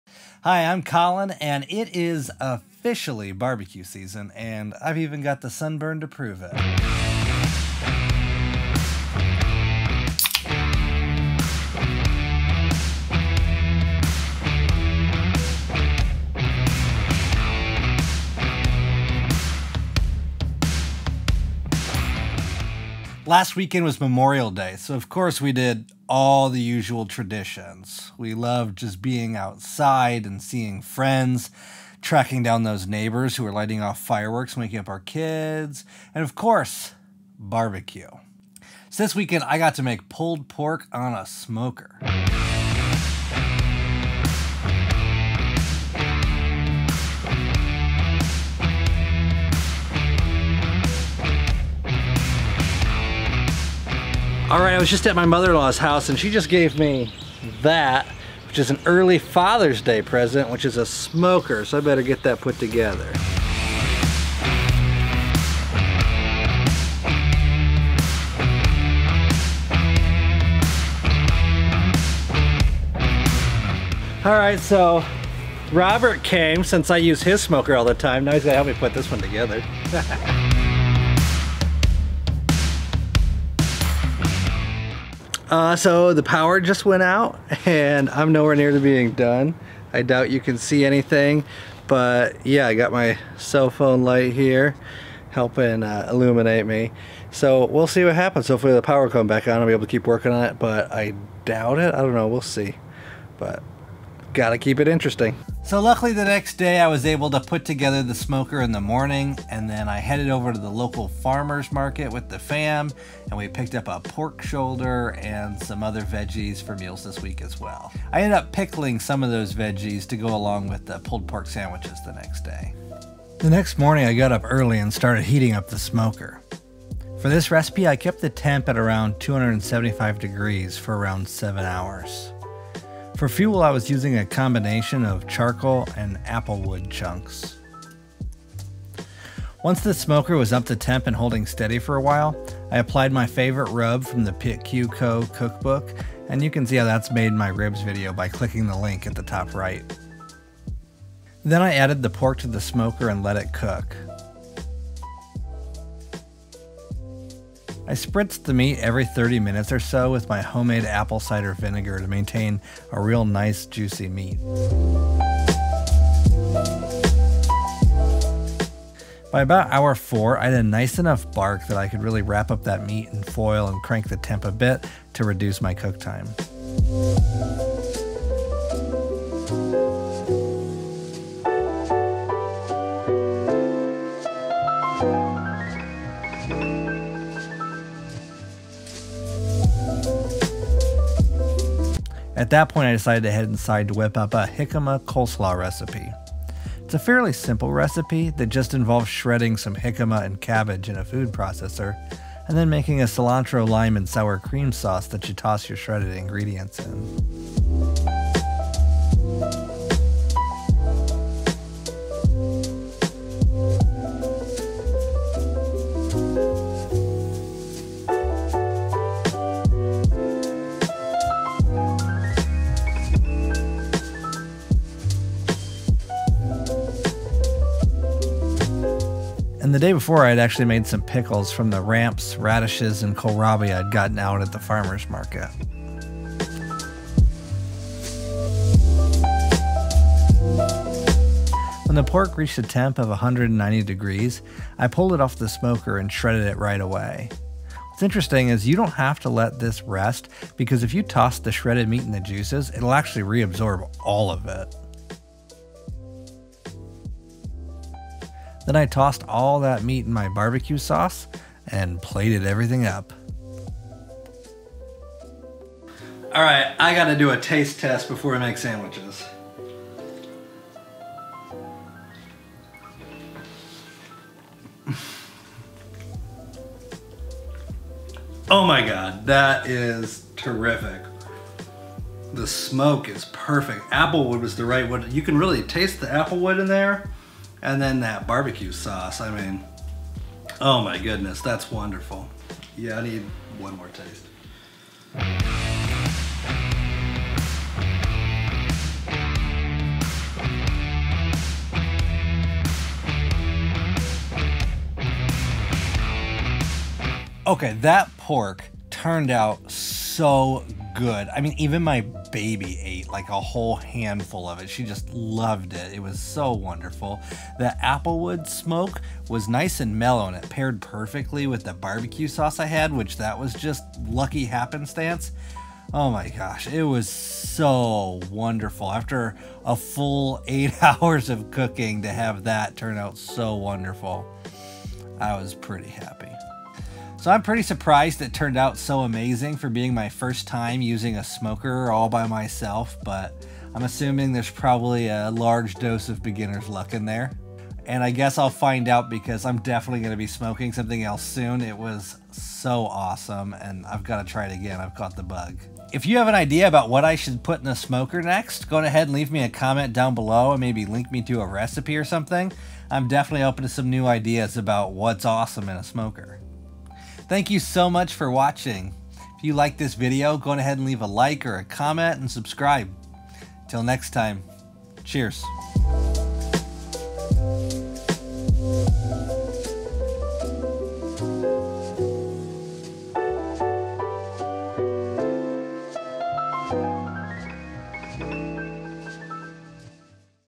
BBQ Pulled Pork Sandwiches on a Smoker
For Memorial Day weekend I whipped up some BBQ Pulled Pork Sandwiches on my new smoker!
This was my first time smoking anything, and I have a lot to learn, but I LOVE IT! I've officially caught the bug!

BBQ Rub Recipe:
1 1/2 tbsp fennel seeds
1 tsp cumin seeds
1tsp black peppercorns
1tsp coriander seeds
1/2 cup packed soft dark brown sugar
1/4 cup granulated sugar
1 tbsp garlic powder
1/3 cup fine salt
2 tbsp smoked paprika
1/4 cup paprika
1 tsp dried oregano
1tsp cayenne
Toast the fennel seeds, cumin seeds, peppercorns and the coriander seeds in a dry pan over medium heat for a few minutes. Allow to cool. Then, blitz toasted spices in a spice grinder. combine with the remaining ingredients, mix thoroughly.